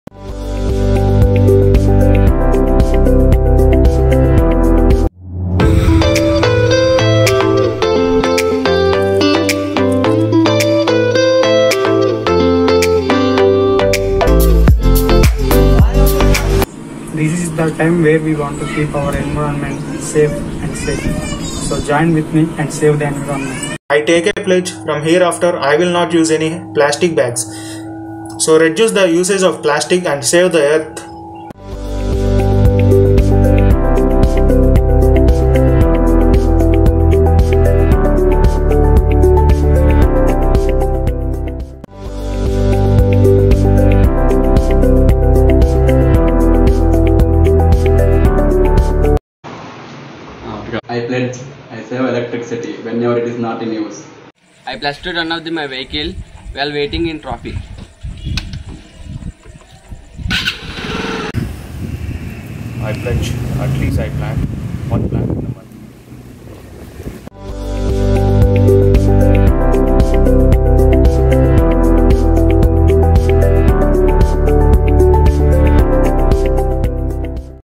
Chennais Amirta Celebrates World Environment Day!
Our students took pledges on world environment day like saving electricity, reducing water wastage, planting trees and more. Chennais Amirta instills moral values in students and we always aim to develop positive character traits.